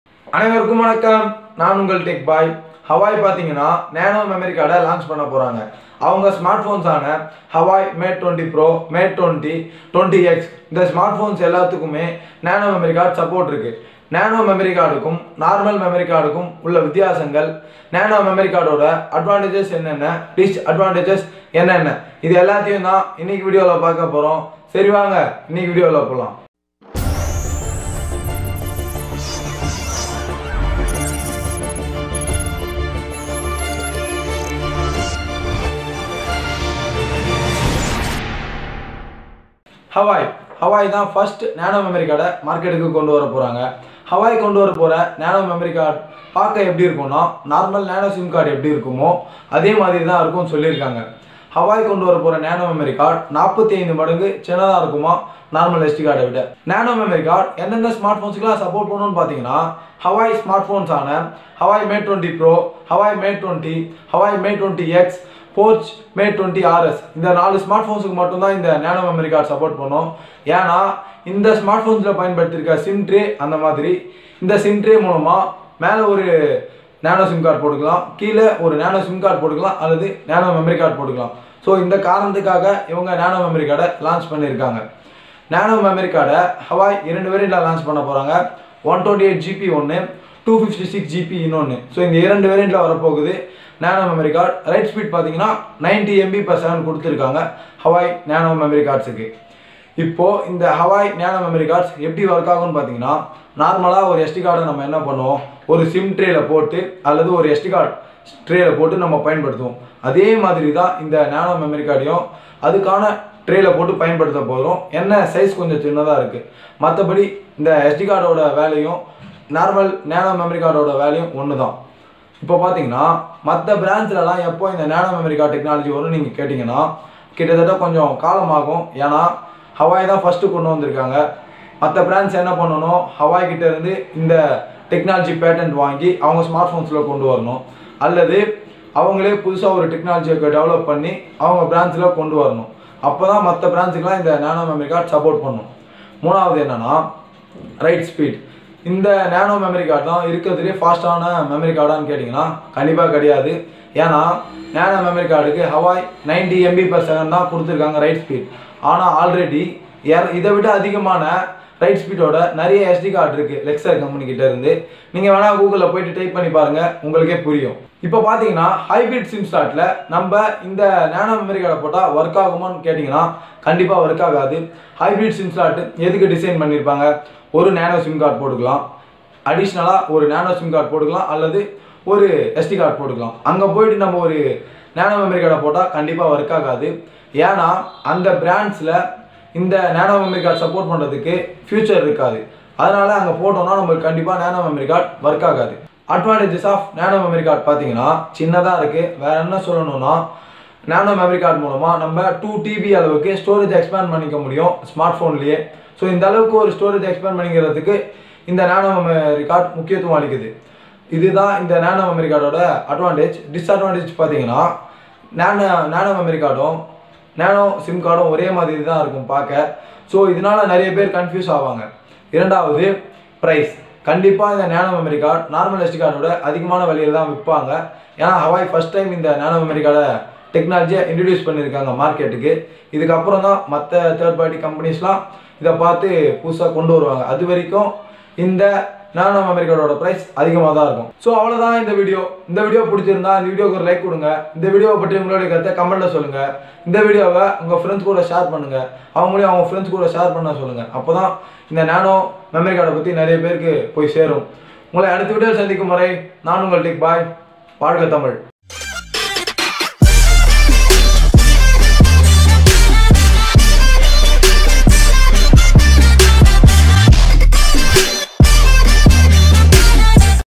Huawei Nano Memory card(NM)Explained in tamil || Tech boy
Huawei nano memory card
It is also type of expandable storage.huawei is first brand to introduce nano memory card to market and it launches huawei mate 20 pro, 2o,20x,porsche mate 20 Rs which supports nano memory card. nano memory card look like a nano sim card and are 45% smaller than normal sd card.huawei is going to launch nano memory card in two variants 128gb,256gb with write speed of 90MB/s.
huawei smartphones only support nano memory card but other brands will take some time to produce smartphones which supports nano memory card.because OEM want to get patent of this technology from huawei to use in their smartphones.nano memory card does not work in hybrid sim slots. nano memory card is not faster than normal sd card because there are many sd cards which have more write speed than nano memory card.
advantages of nano memory card
1 it is small
2 using  nano memory card we can expand storage upto 2tb.
disadvantages of nano memory card
1 nano sim card and nano memory card looks similar sometimes it can be confused.
2 price of  nano memory card will be higher than normal sd card because nano memory card support huawei smartphones only support nano memory card but other brands will take some time to produce smartphones which supports nano memory card . these are advantages and disadvantages of nano memory card.